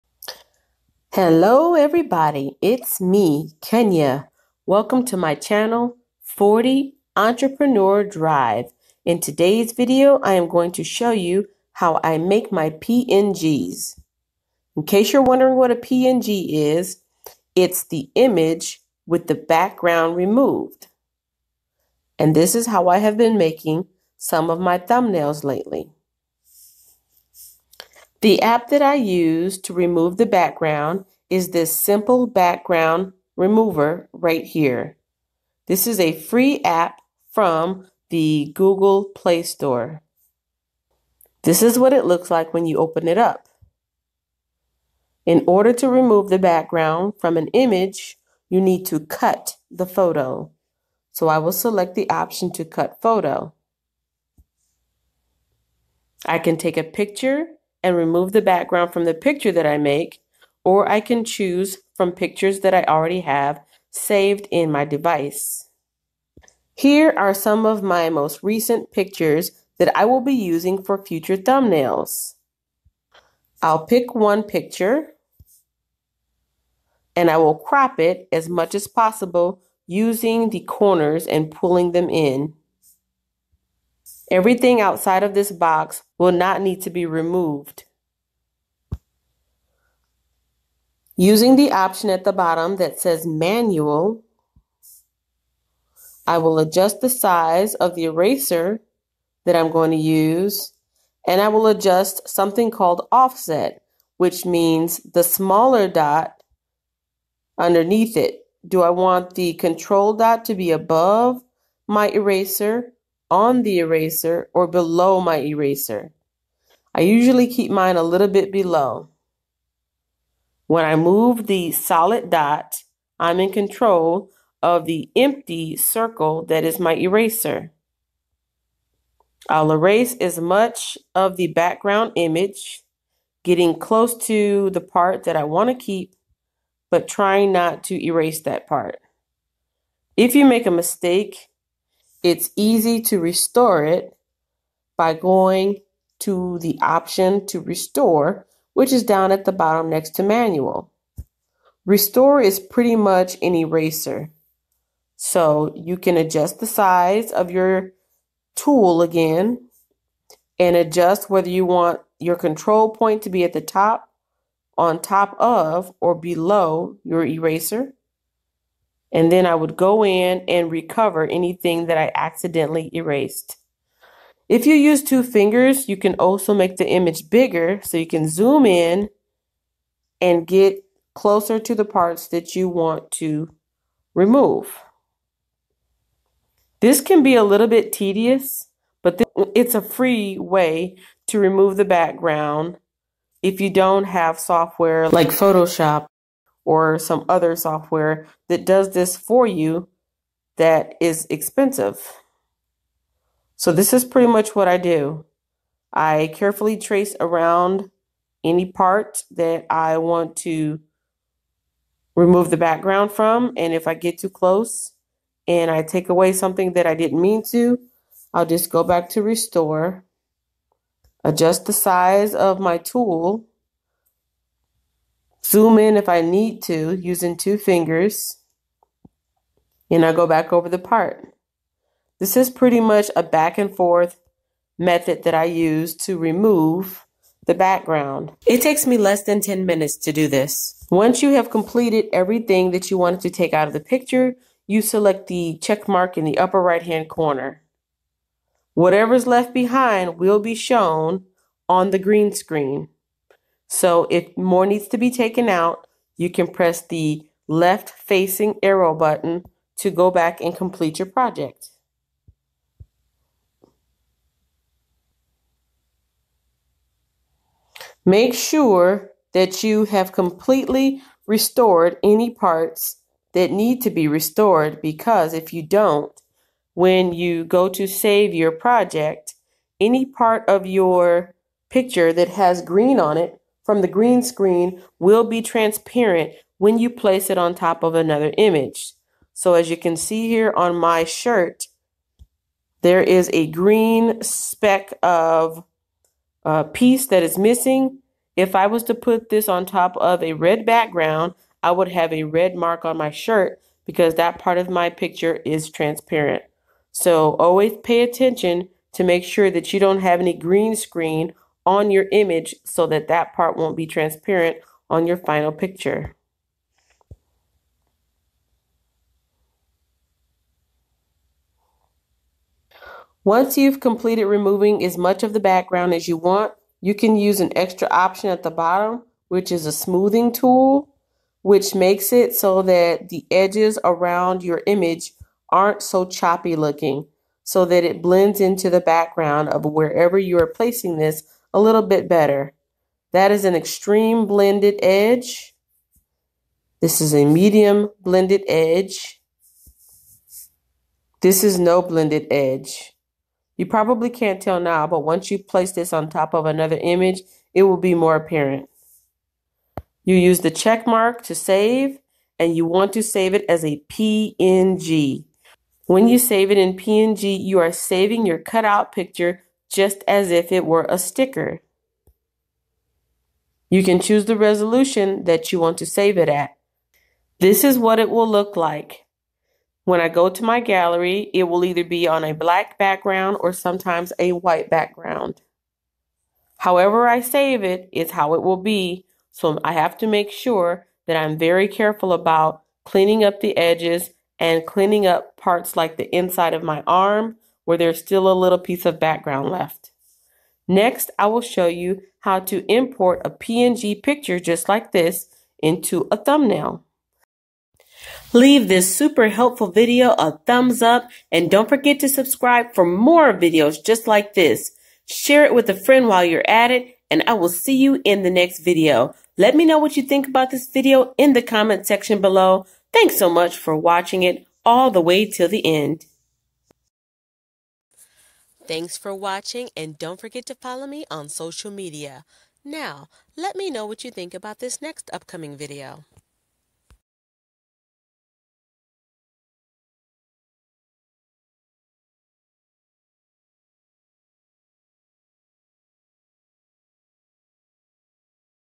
How to make your own sticker to put on your thumbnails
This is how I remove the background from pictures that I take to make a sticker out of them.
It is also called a PNG, or portable Network graphic.
I made a video on how to use PNG/Stickers in your thumbnails. You can watch it here:
The name of the app I am using is Simple Background Changer, by Photo Cool Apps
There is a free and paid version. I downloaded this from the Google Play store.